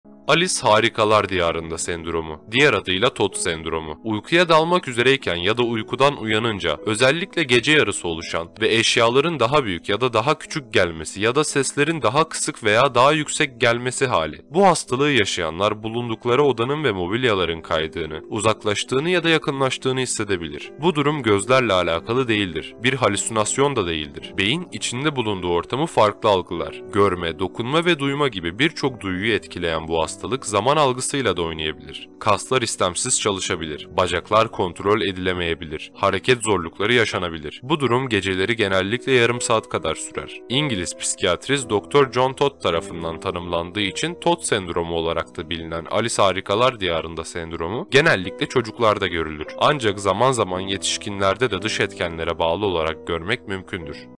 Alice Harikalar Diyarında Sendromu ?
"Garip ve Ürkütücü Psikiyatrik Rahatsızlıklar !" isimli videomuzda hem garip psikiyatrik rahatsızlıklardan hem de ürkütücü psikiyatrik rahatsızlıklardan bahsettik. Bu videoda ise Alice Harikalar Diyarında Sendromu nedir sorusuna cevap verdik. İyi seyirler dilerim.

Radyoutube diye de biliriz, diyedebiliriz.